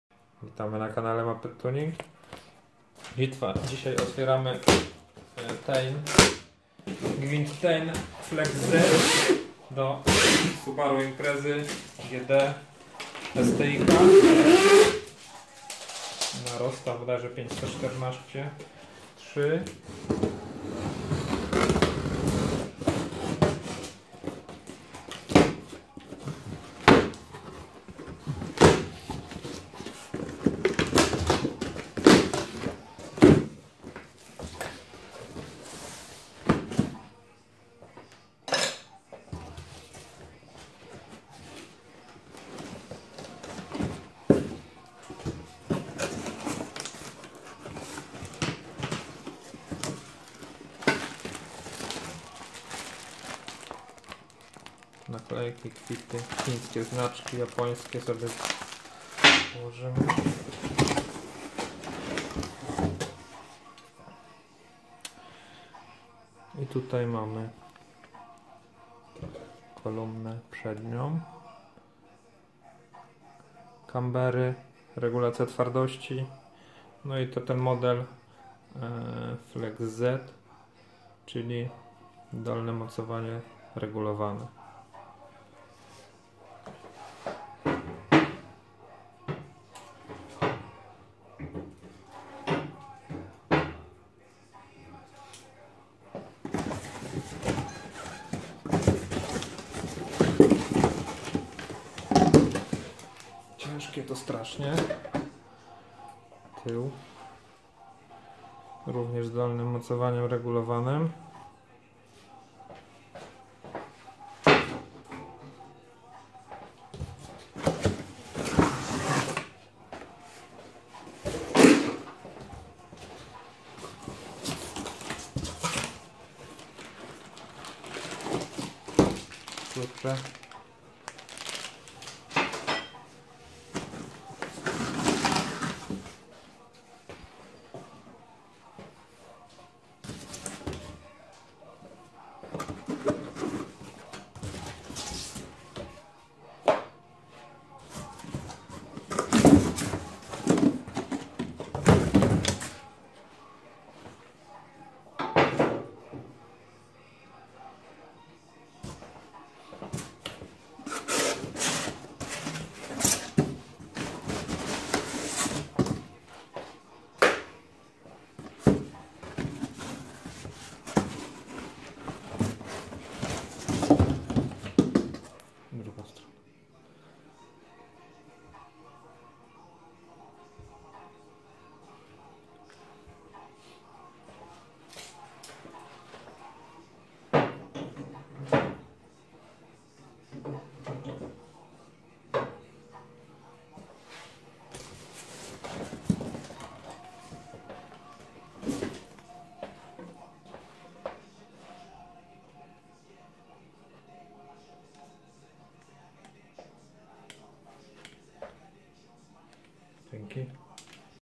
Zawieszenie Gwintowane TEIN Flex Z Subaru Impreza WRX STI - MAPET-TUNING UNBOXING
Gwint TEIN DO subaru Imprezy w wersji Flex Z
Regulacja Twardości | Regulowane dolne mocowania | Camber Plates
Pełna oferta TEIN u nas w sklepie - Zapraszamy